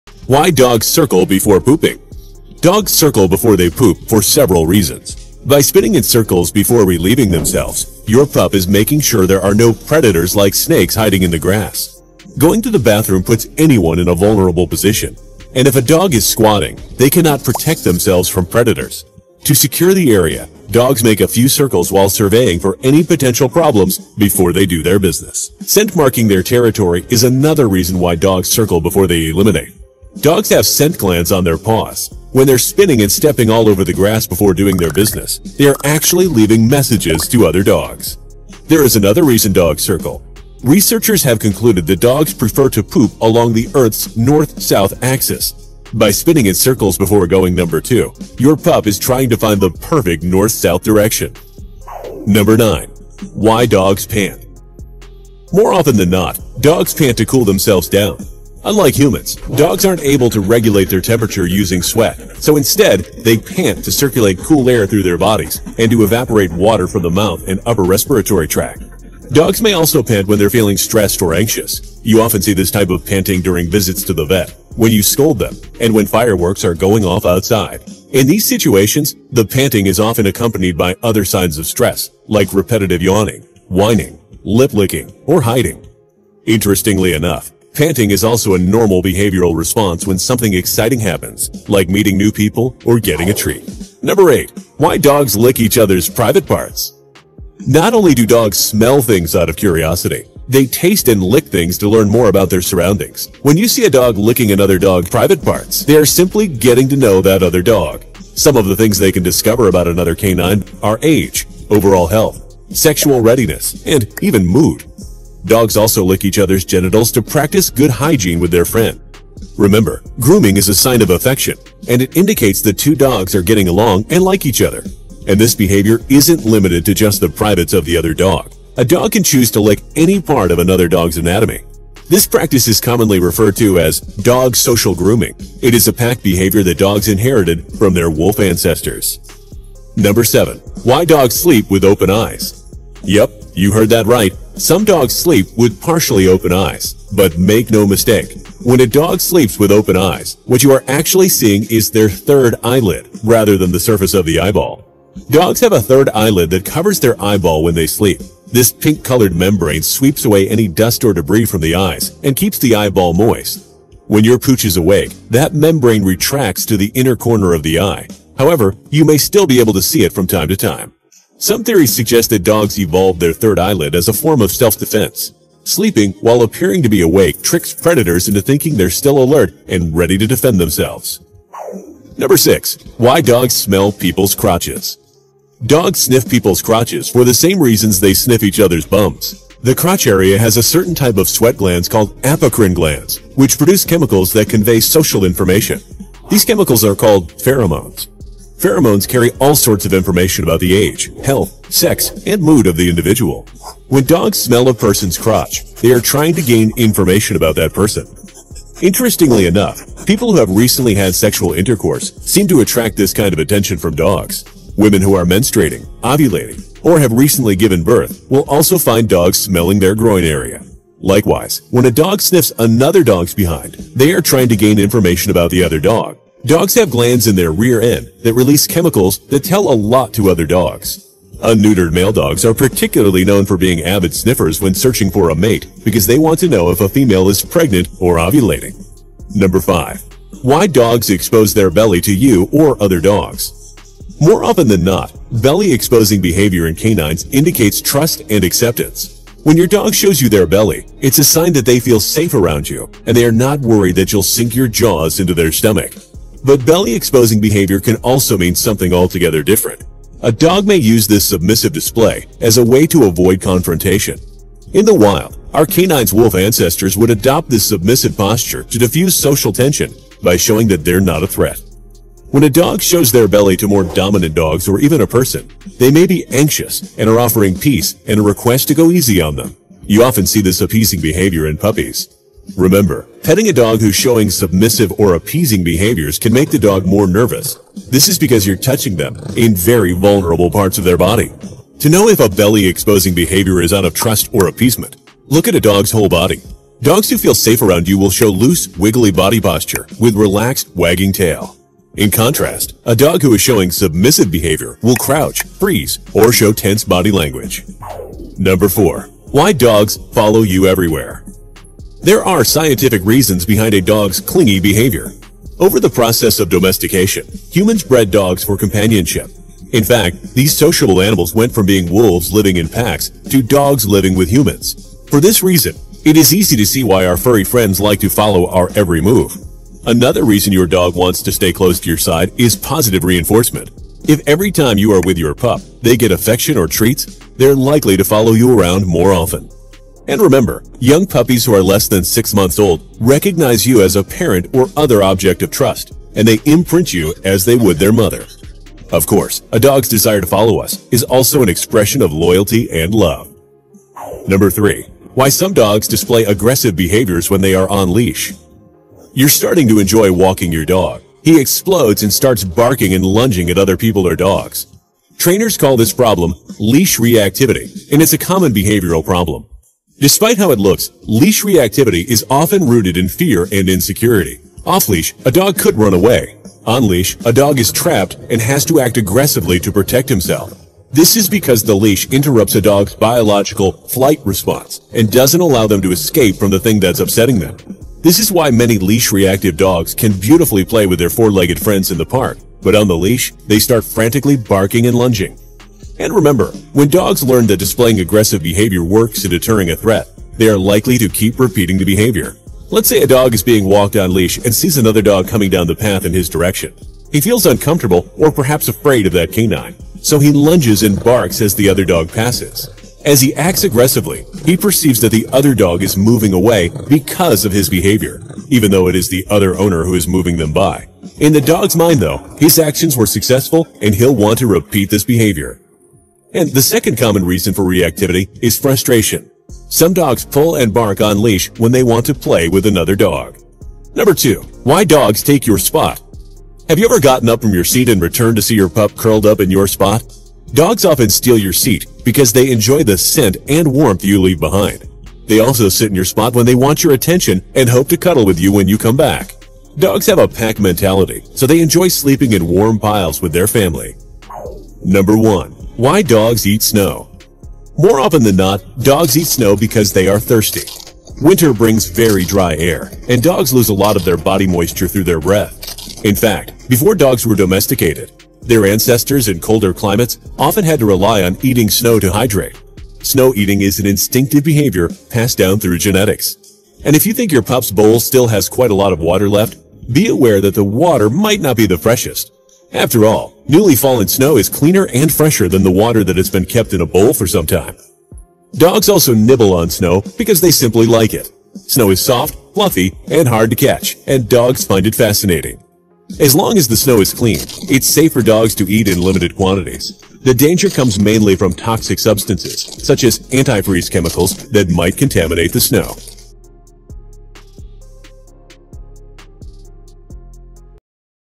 11 Odd Dog Behaviors and What They Actually Mean / sniffing crotch - circle before pooping - exposin
11 Odd Dog Behaviors and What They Actually Mean / sniffing crotch - circle before pooping - exposing belly